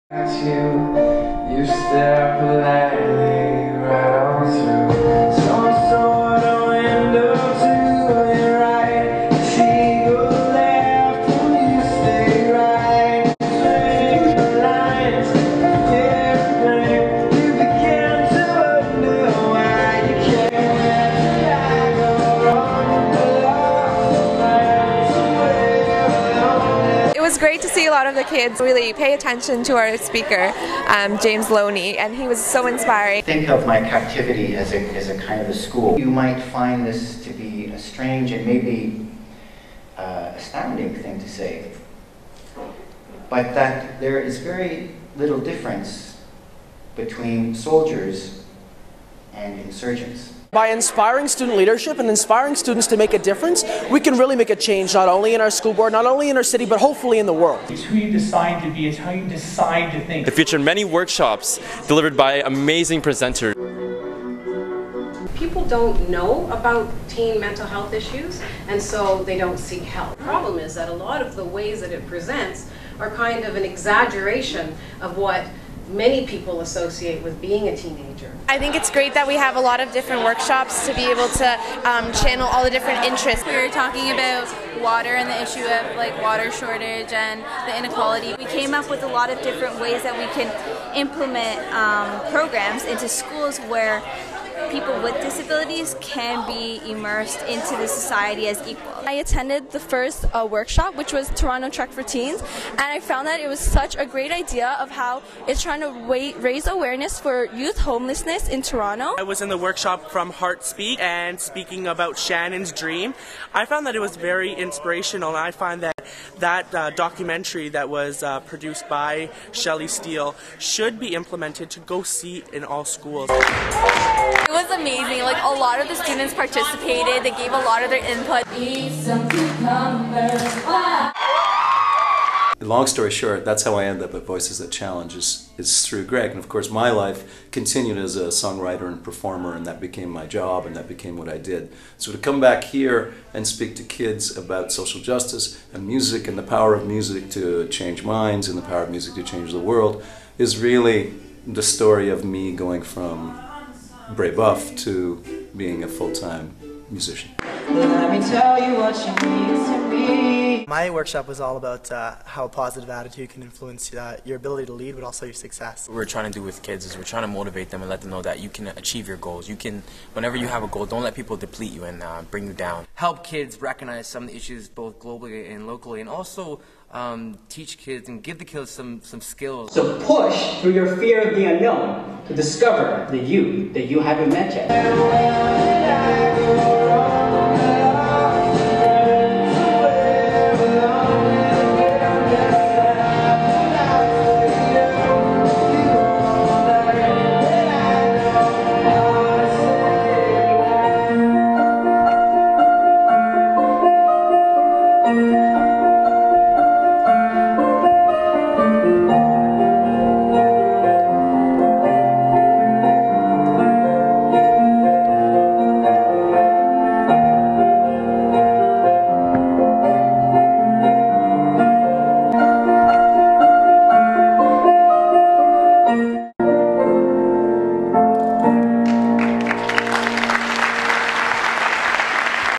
TCDSB's Voices that Challenge Student Leadership Conference 2012
This video by Heartspeak features highlights of the TCDSB's Voices that Challenge 2012 Student Leadership Conference. Themes addressed include Social Justice, Mental Wellness, Inclusion and Belonging.  The conference was hosted by the Catholic Student Leadership Impact Team (CSLIT) and Tom Barlow, Juno Nominee and Much Music Award recipient.